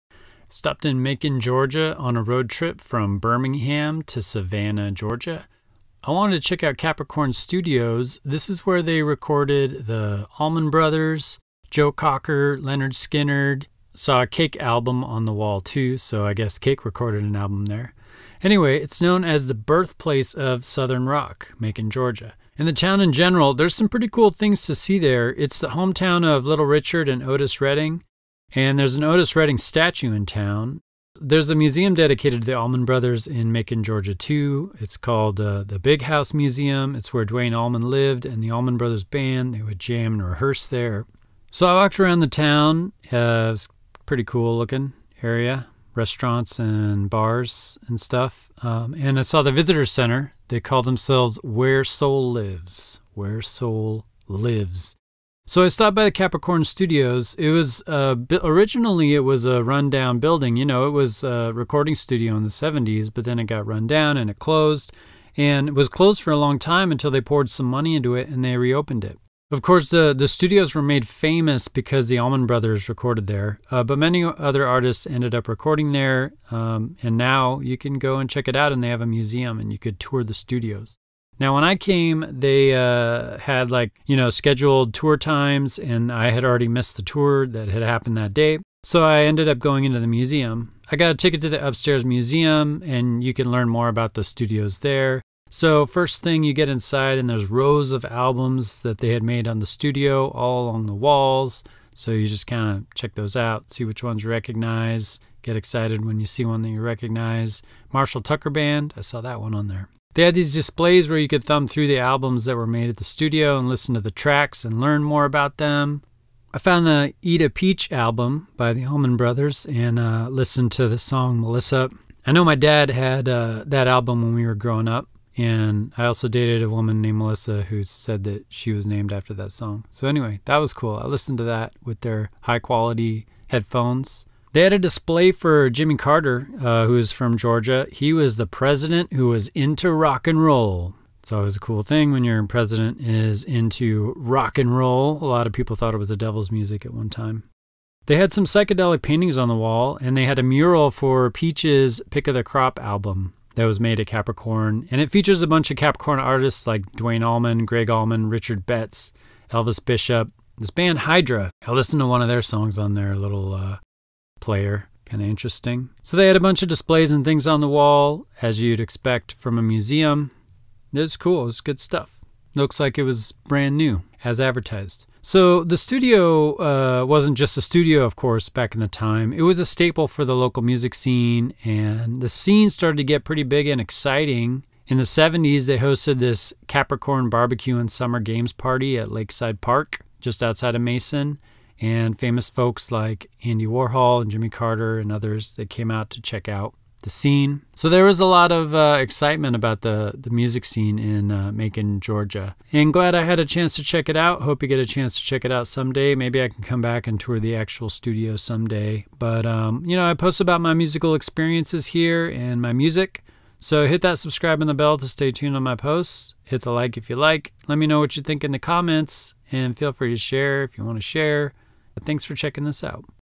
A visit to the home of southern rock (musical landmarks in Macon, Georgia)
I stopped by Macon Georgia on a road trip from Birmingham Alabama to Savannah Georgia. I was on the lookout for musical landmarks and musical signts in Georgia.

Macon, Georgia calls themselves the city "where soul lives" and they're the birthplace of Otis Redding and Little Richard. They have a statue of Otis Redding in the city and an Allman Brothers Museum called the Big House.

I toured Capricorn Studios, which is an old recording studio that was recently renovated and re-opened for tours. They have a nice museum inside where they feature the famous bands that recorded there, including the Allman Brothers, Marshall Tucker Band and more. They call themselves the birthplace of southern rock.